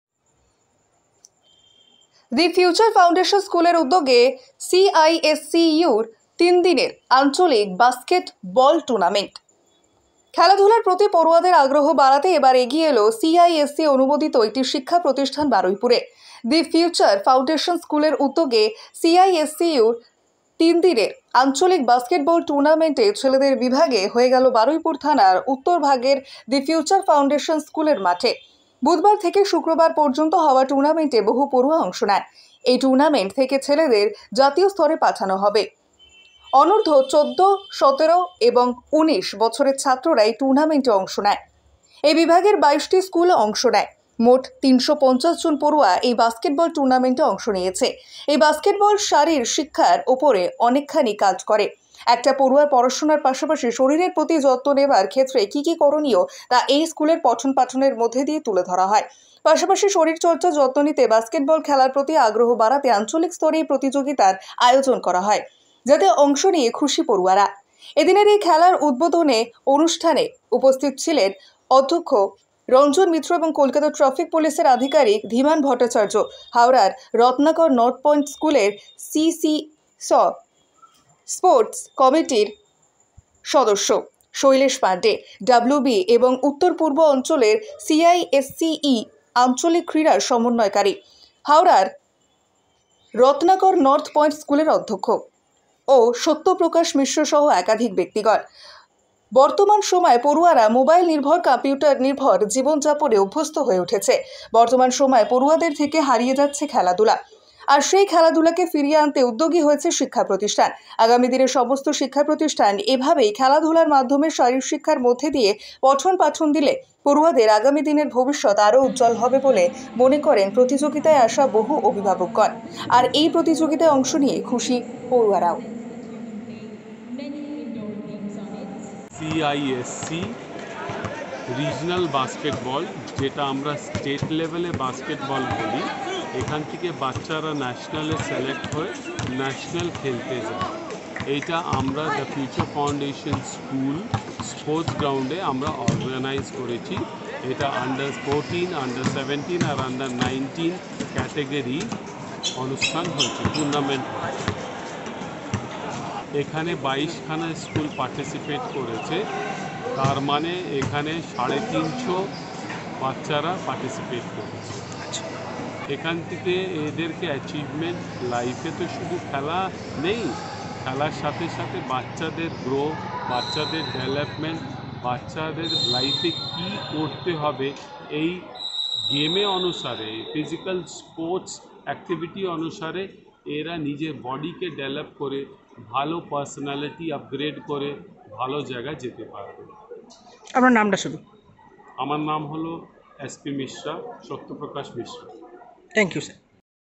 দি ফিউচার ফাউন্ডেশন স্কুলের উদ্যোগে সিআইএসসিইর তিন দিনের আঞ্চলিক বাস্কেট বল টুর্নামেন্টে
কলকাতা অফিস : খেলাধুলার প্রতি পড়ুয়াদের আগ্রহ বাড়াতে এবারে এগিয়ে এলো সিআইএসসি অনুমোদিত একটি শিক্ষা প্রতিষ্ঠান বারুইপুরে। দি ফিউচার ফাউন্ডেশন স্কুলের উদ্যোগে সিআইএসসিইর তিনদিনের আঞ্চলিক বাস্কেট বল টুর্নামেন্টে ছেলেদের বিভাগে হয়ে গেল বারুইপুর থানার উত্তরভাগের দি ফিউচার ফাউন্ডেশনে স্কুলের মাঠে। বুধবার থেকে শুক্রবার পর্যন্ত হওয়া টুর্নামেন্টে বহু পড়ুয়া অংশ নেন।এই টুর্নামেন্ট থেকে ছেলেদের জাতীয় স্তরে পাঠানো হবে। অনূর্ধ্ব১৪,১৭ ও ১৯ বছরের ছাত্ররা এই টুর্নামেন্টে অংশ নেয়।এই বিভাগের ২২ টি স্কুল অংশ নেয়। মোট ৩৫০ জন পড়ুয়া এই বাস্কেটবল টুর্নামেন্টে অংশ নিয়েছে। এই বাস্কেটবল শারীর শিক্ষার ওপরে অনেকখানি কাজ করে। একটা পড়ুয়ার পড়াশোনার পাশাপাশি শরীরের প্রতি যত্ন নেওয়ার ক্ষেত্রে কি কি করনীয় তা এই স্কুলের পঠন-পাঠনের মধ্যে দিয়ে তুলে ধরা হয়, পাশাপাশি শরীরচর্চার যত্ন নিতে বাস্কেটবল খেলার প্রতি আগ্রহ বাড়াতে আঞ্চলিক স্তরে এই প্রতিযোগিতার আয়োজন করা হয়। যাতে অংশ নিয়ে খুশি পড়ুয়ারা। এ দিনের এই খেলার উদ্বোধন অনুষ্ঠানে উপস্থিত ছিলেন অধ্যক্ষ রঞ্জন মিত্র ও কলকাতা ট্রাফিক পুলিশের আধিকারিক,ধীমান ভট্টাচার্য হাওড়ার রত্নাকর নর্থ পয়েন্ট স্কুলের সিসিস স্পোর্টস কমিটির সদস্য। শৈলেশ পান্ডে, পশ্চিমবঙ্গ এবং উত্তর-পূর্ব অঞ্চলের CISCE আঞ্চলিক ক্রীড়া সমন্বয়কারী। হাওড়ার রত্নাকর নর্থ পয়েন্ট স্কুলের অধ্যক্ষ ও সত্য প্রকাশ মিশ্র সহ একাধিক ব্যক্তিত্বগণ। বর্তমান সময়ে পড়ুয়ারা মোবাইল নির্ভর, কম্পিউটার নির্ভর জীবন যাপনে অভ্যস্ত হয়ে উঠছে। বর্তমান সময়ে পড়ুয়াদের থেকে হারিয়ে যাচ্ছে খেলাধুলা, আর সেই খেলাধুলাকে ফিরিয়ে আনতে উদ্যোগী হয়েছে শিক্ষা প্রতিষ্ঠান। আগামী দিনে সমস্ত শিক্ষা প্রতিষ্ঠান এইভাবে খেলাধুলার মাধ্যমে শারীর শিক্ষার মধ্যে দিয়ে পঠন পাঠন দিলে পড়ুয়াদের আগামী দিনের ভবিষ্যৎ আরো উজ্জ্বল হবে বলে মনে করেন প্রতিযোগিতায় আসা বহু অভিভাবকগন। আর এই প্রতিযোগিতায় অংশ নিয়ে খুশি পড়ুয়ারা।